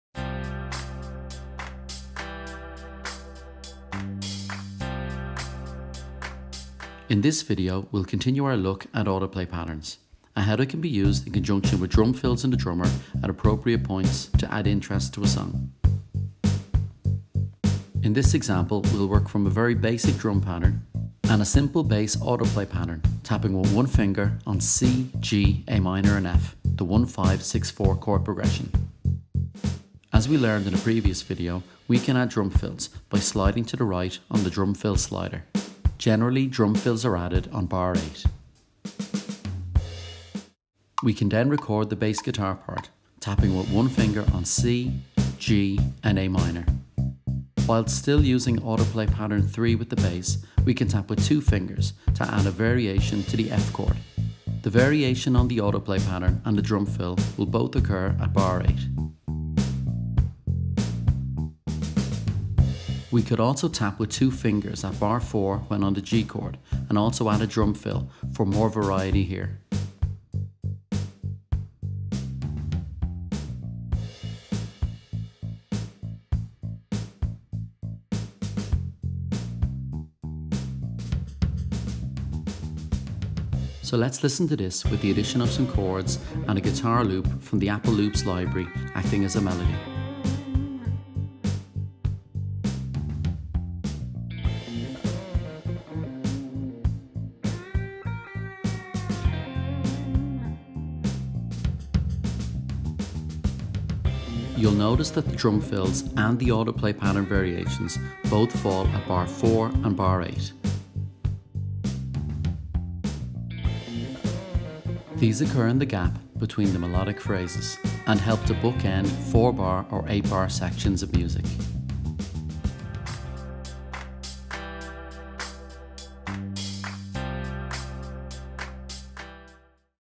Day 12 - 90 Days of GarageBand. Using Autoplay Patterns in conjunction with Drum Fills
90 Days of GarageBand - Day 12.
Using Autoplay Patterns in conjunction with Drum Fills at bars 4 and 8 to bookend phrases and/or sections of a song with GarageBand on iPad.